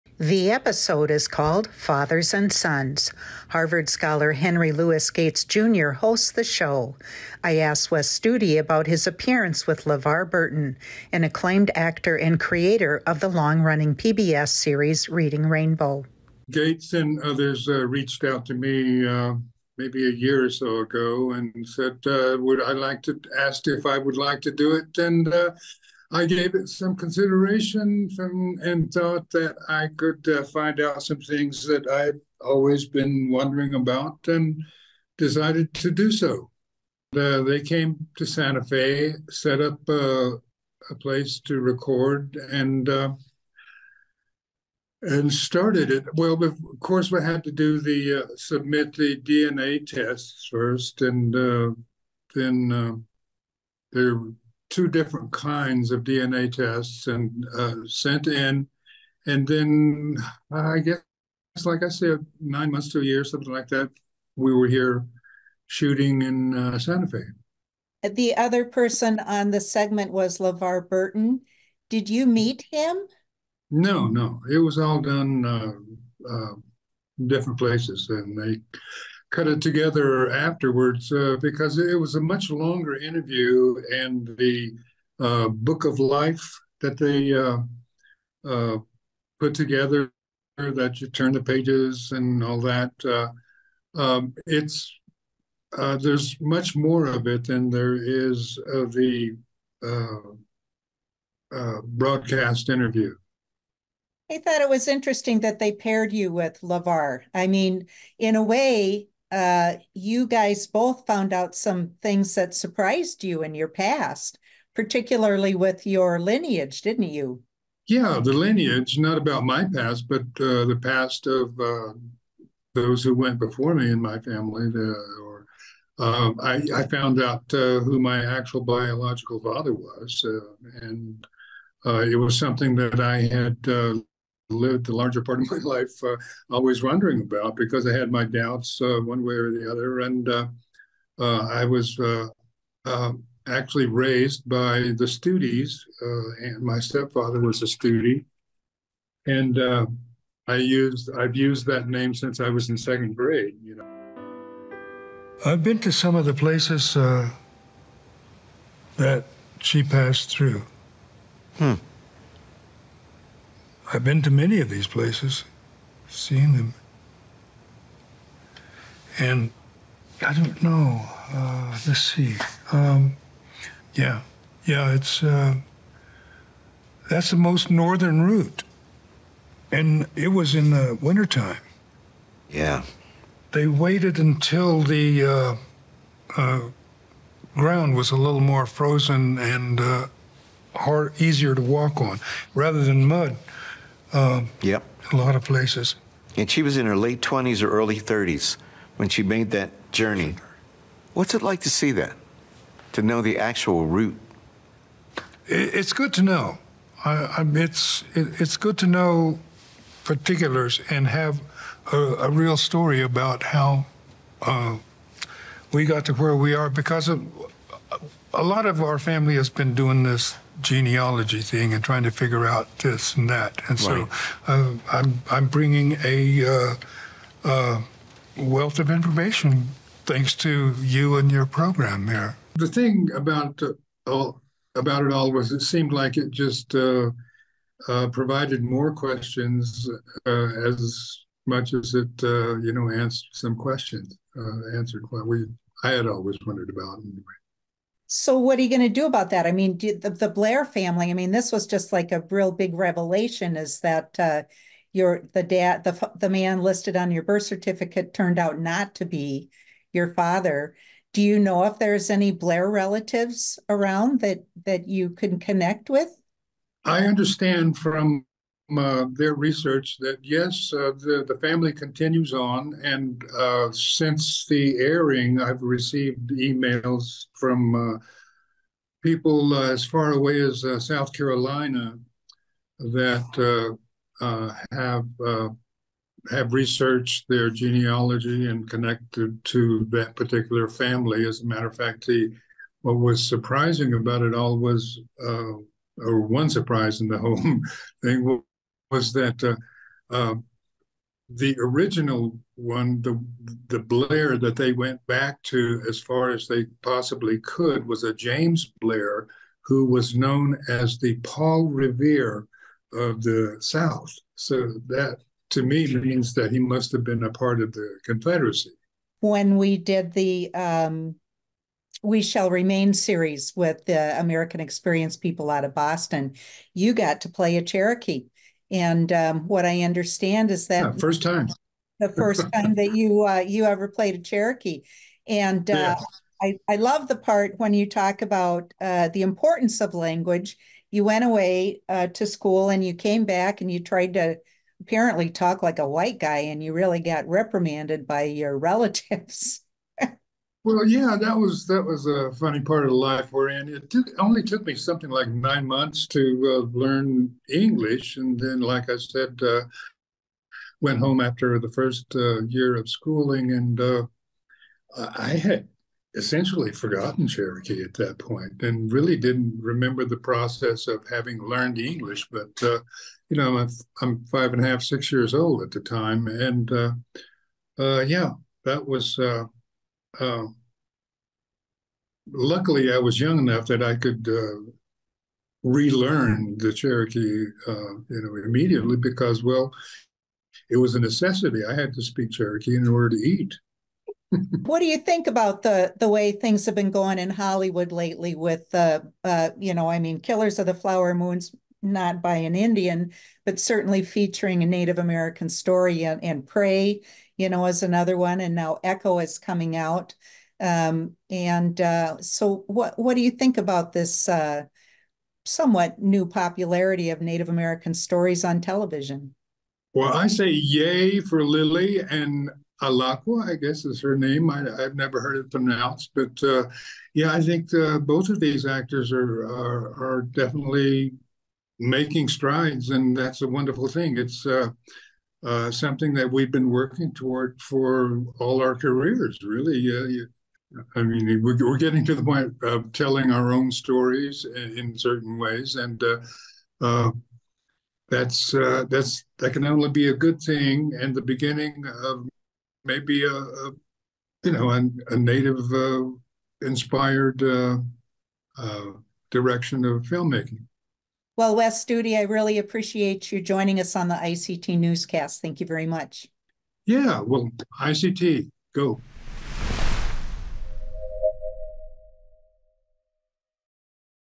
Wes Studi discovers his roots
Indian Country Today's daily newscast
Award-winning Cherokee actor, Wes Studi discovers some surprising facts about his ancestry on 'Finding Your Roots.' ICT's Shirley Sneve has this interview: